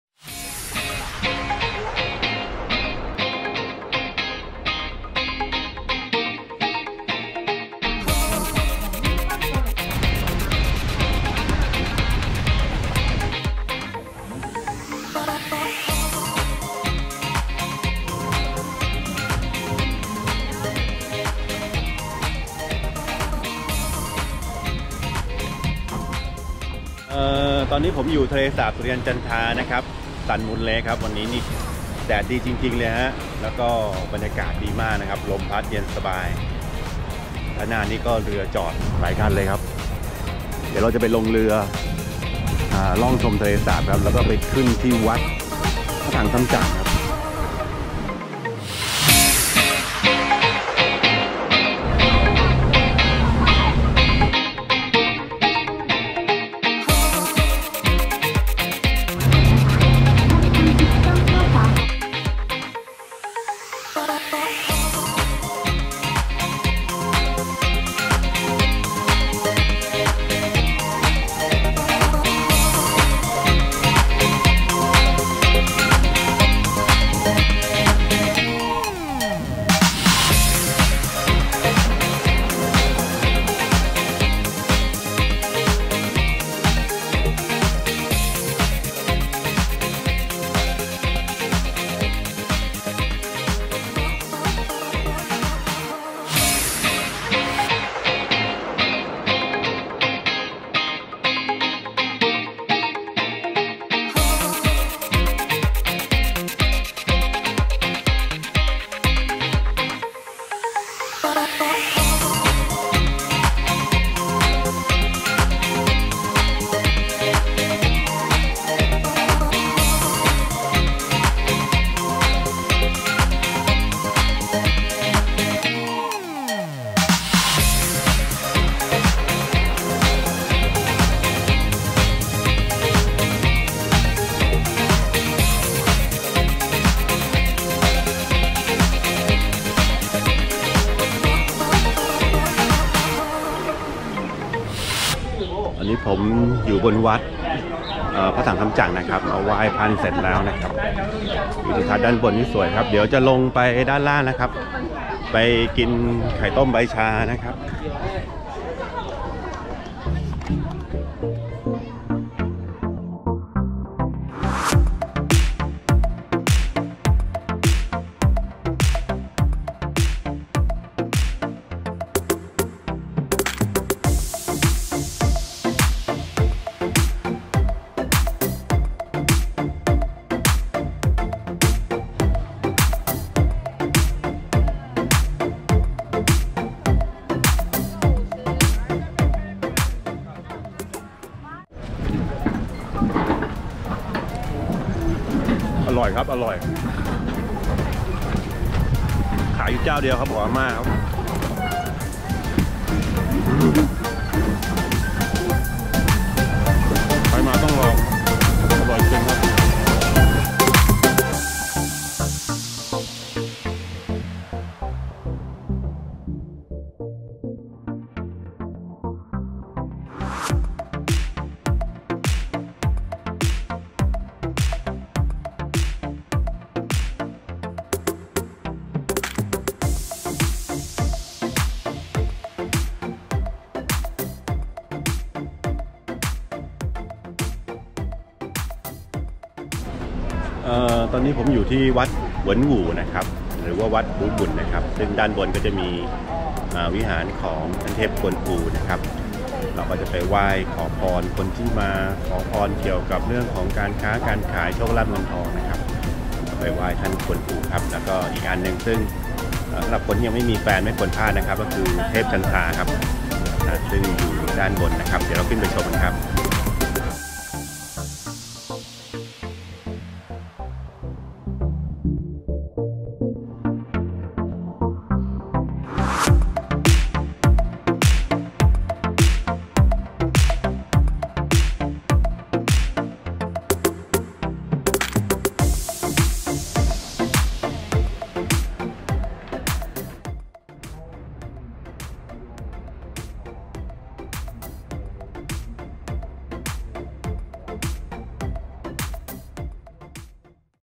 ทะเลสาบ สุริยัน จันทรา Sunmoon lake Nov 2019
ถือว่าเป็นทะเลสาบแห่งเดียวใน ไต้หวัน เป็นสถานที่ท่องเที่ยวขึ้นชื่อ มีวัดพระถังซัมจั๋ง และวัดหวินวู่ อยู่บริเวณใกล้ๆ
     สวัสดีครับทุกท่าน ผมจะทำคลิปออกมาให้ชมเรื่อยๆ เพื่อให้เห็นสถานที่ ท่องเที่ยวต่างๆที่อยู่ในความสนใจของทุกท่านครับ ขอบคุณครับ

ติดตามทางเพจ พี่ขวดพาเที่ยว